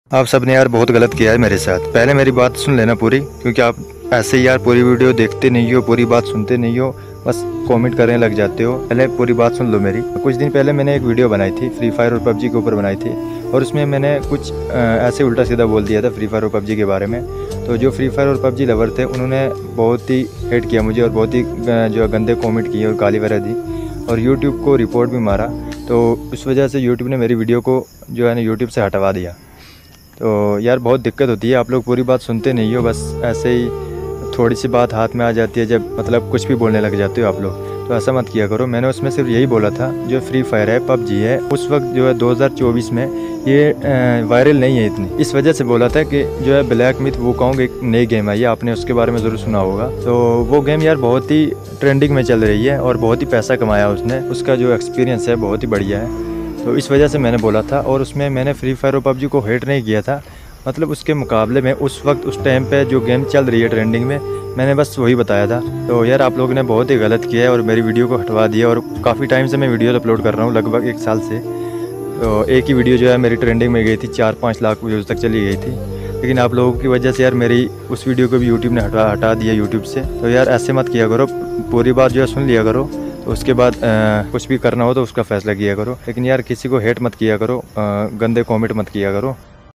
Bad Habits of Free Fire & PUBG Lovers Exposed"😡
In this video/article,
we explore the common bad habits many Free Fire and PUBG players develop while indulging in their favorite games. From excessive screen time to neglecting real-life responsibilities, we address the unhealthy behaviors that can arise from addictive gaming. Whether you're a Free Fire fanatic or a PUBG lover, it's time to face the truth and break these habits for a healthier gaming experience!